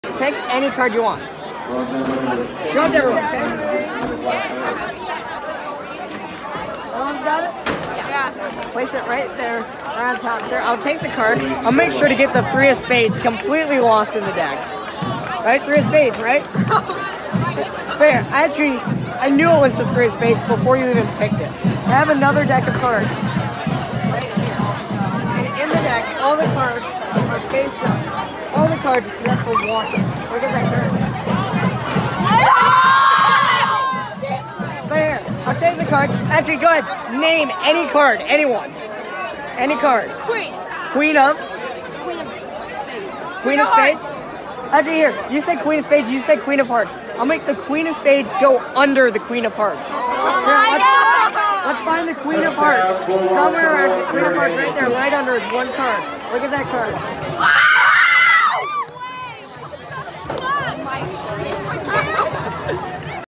Last Football Magic For The Year!
This is my last time doing football magic until next year. This is also one of my favorite tricks to do for crowds.